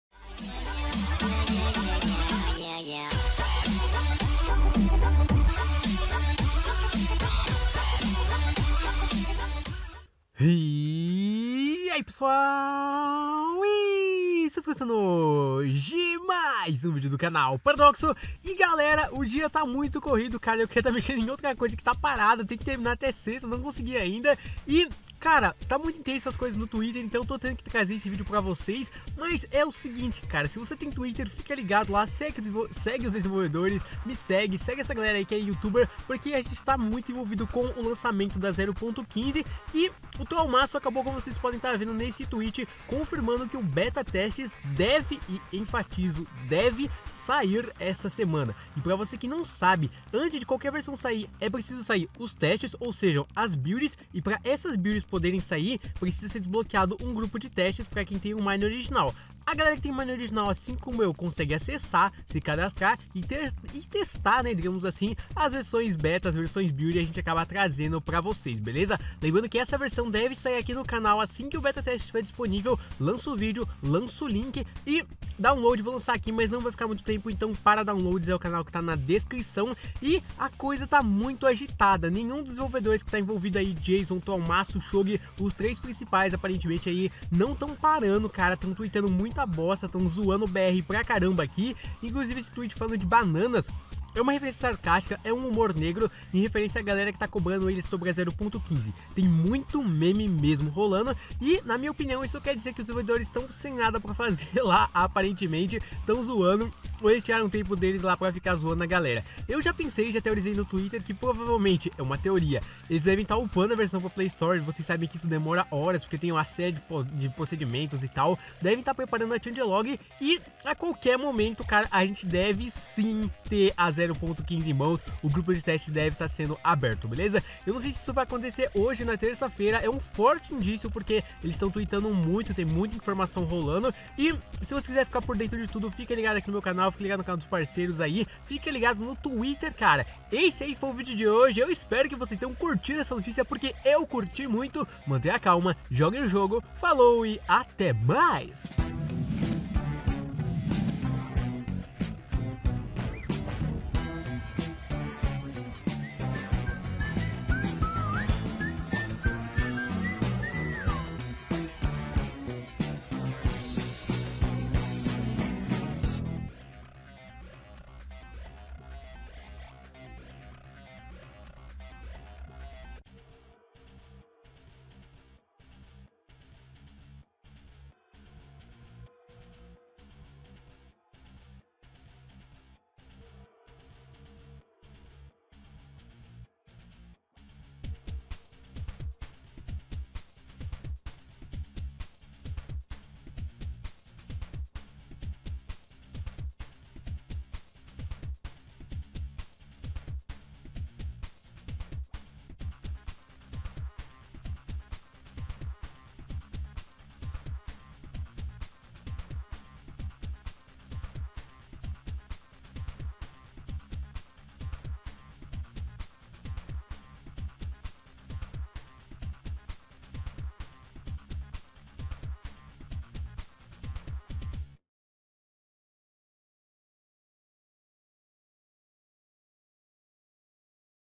MCPE 0.15.0 News: Versão poder ser lançada a qualquer momento! ENTENDA
O beta testes vai ser aberto nos próximos dias, mas isso não é tudo. A Build 1 do Minecraft PE 0.15 já pode estar na PlayStore. Espero que curtam jovens.